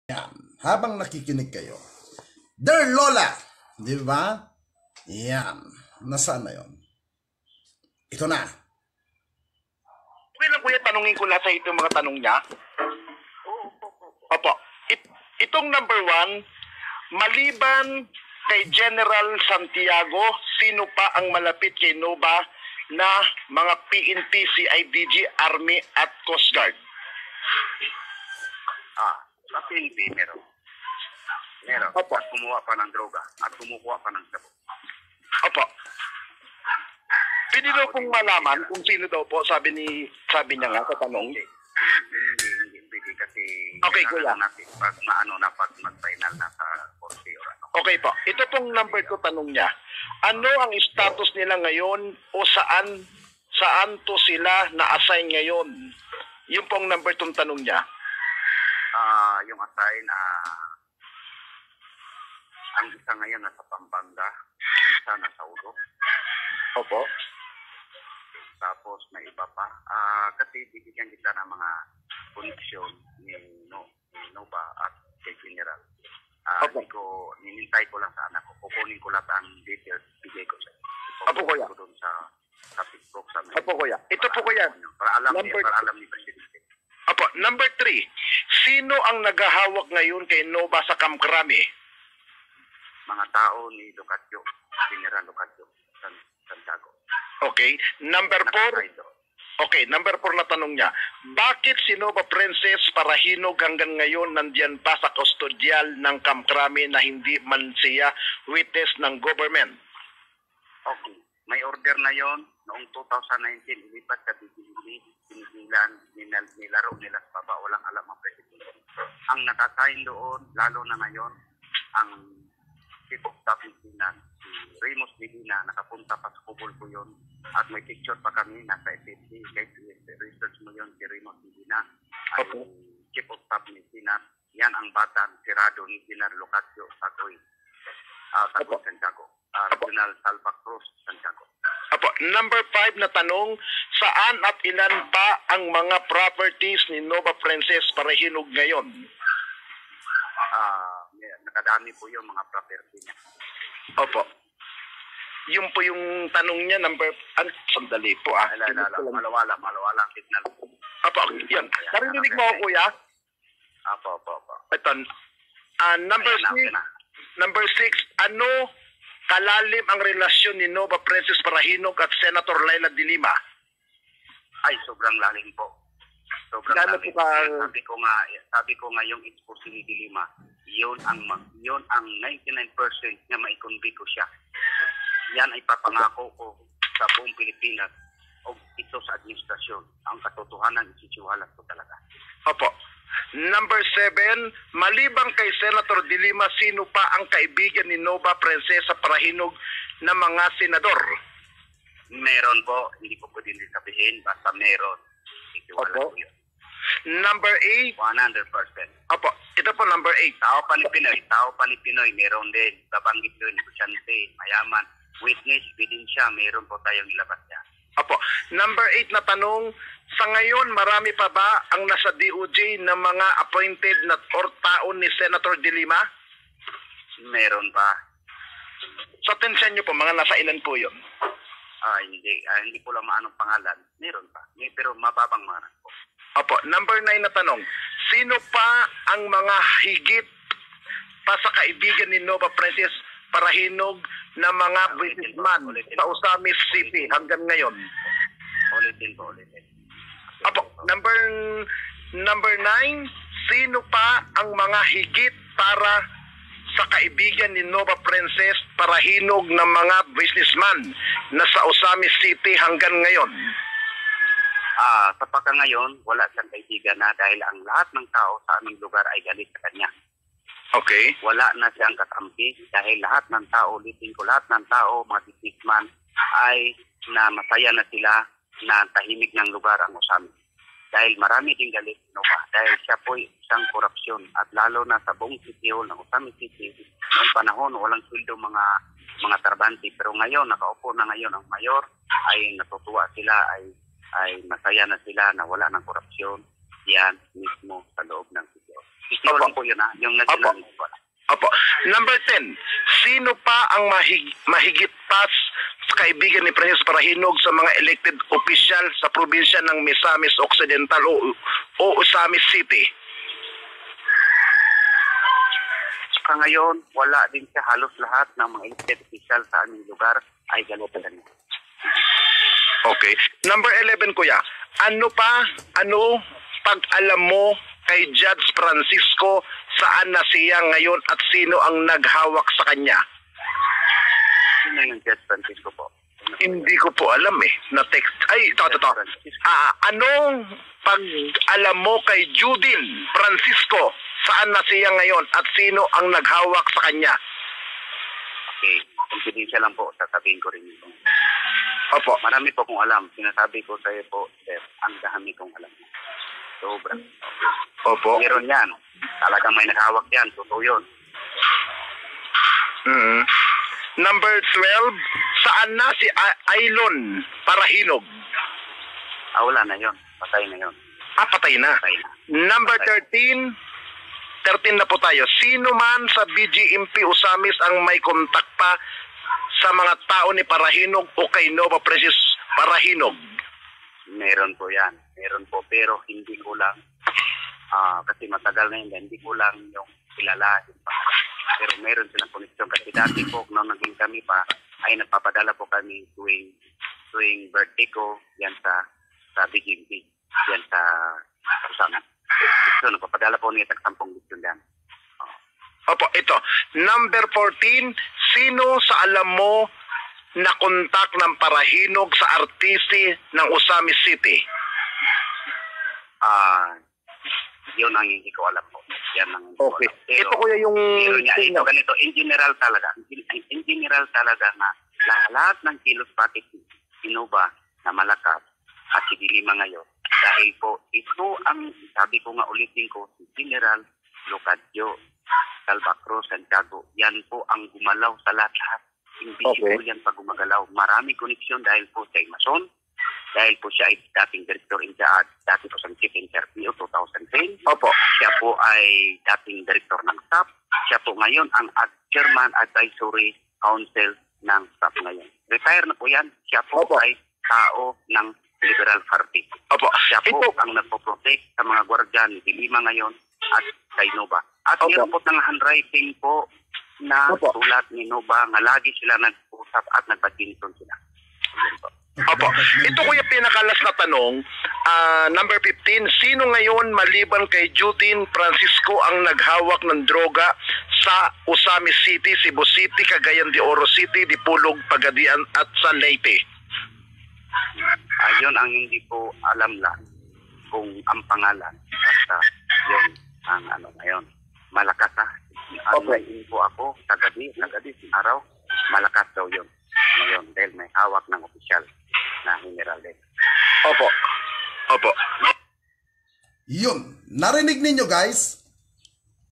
Herbert Colangco idiniin lalo si Sen. Laila Delima at isinabit Ang isang General?
Heto ang exclusive interview ni Niño Barzaga kay Herbert Colangco Laila Delima idiin lalo..

Ctto...